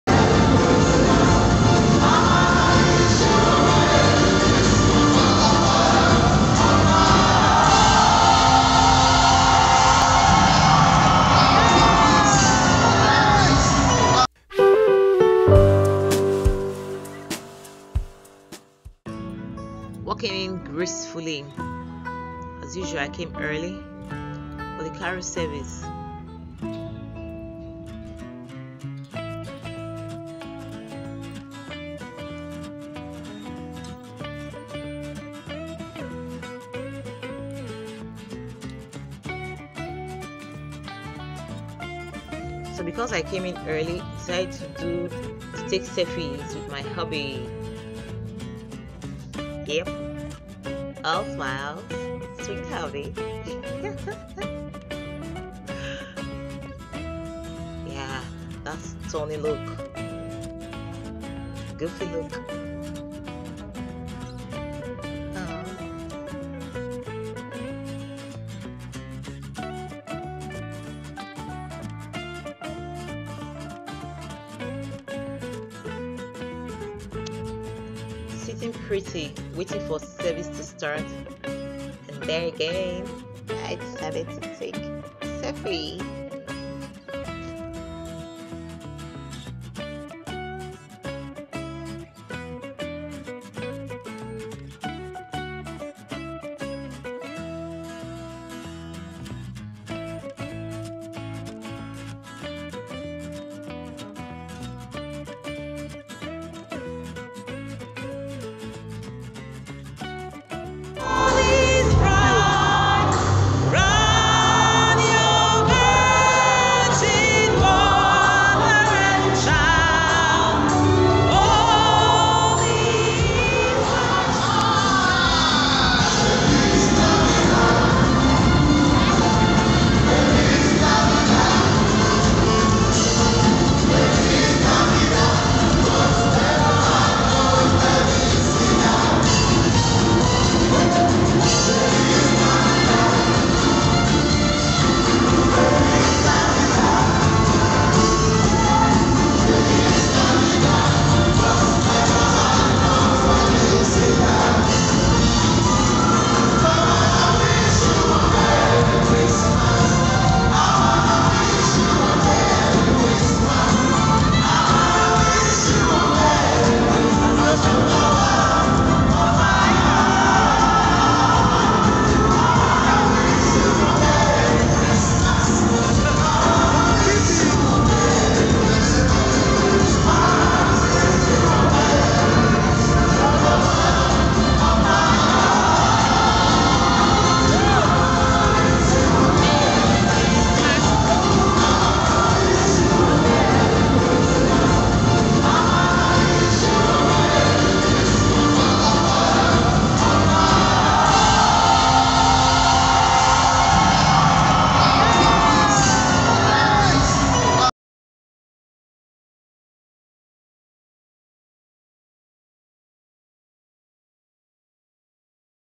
Vlog/ Christmas Carol Service
In this vlog, you will be carried along a fun time of a Christmas Carol service.